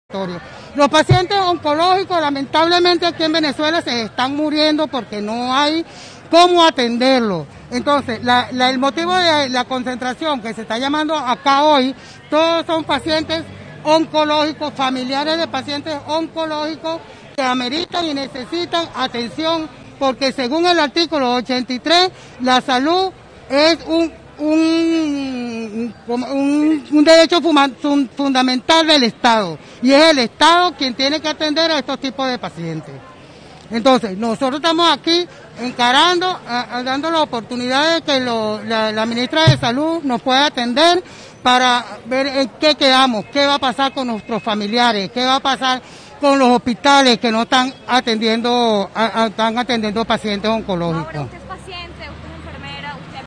DistritoCapital | Pacientes oncológicos manifestaron para exigir atención
Un grupo de pacientes oncológicos protestó este jueves 5 de mayo, en la sede del Ministerio de Salud, ubicada en la avenida Baralt en el municipio Libertador de Caracas.

Los manifestantes le exigieron a las autoridades gubernamentales que solventen la dificultad que existe para adquirir medicamentos. "Los pacientes oncológicos en Venezuela, lamentablemente se están muriendo porque no hay cómo atenderlos", dijo Dayana Briceño, esposa de un paciente, quien aseguró que esperan la atención de la ministra de Salud, Magaly Gutiérrez.

Video: cortesía
Reporte: cortesía